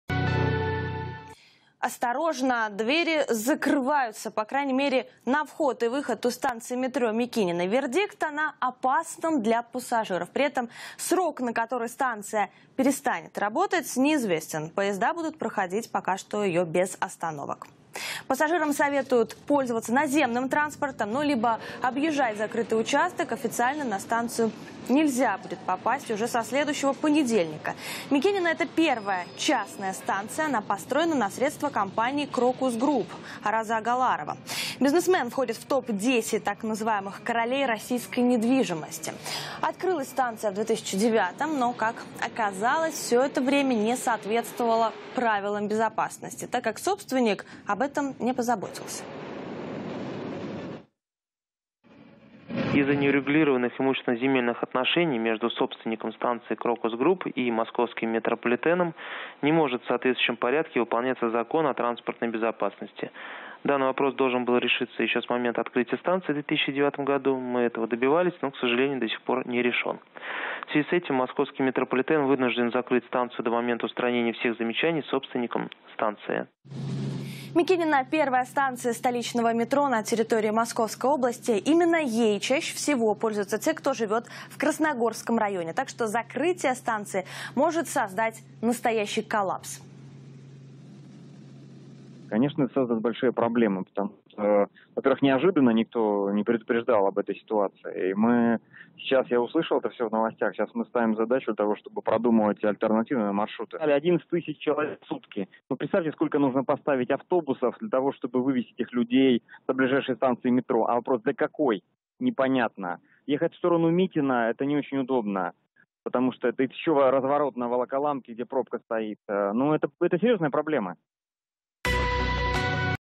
Станция метро "Мякинино" закроют из-за опасности для пассажиров
Станция "Мякинино" будет закрыта на неопределенный срок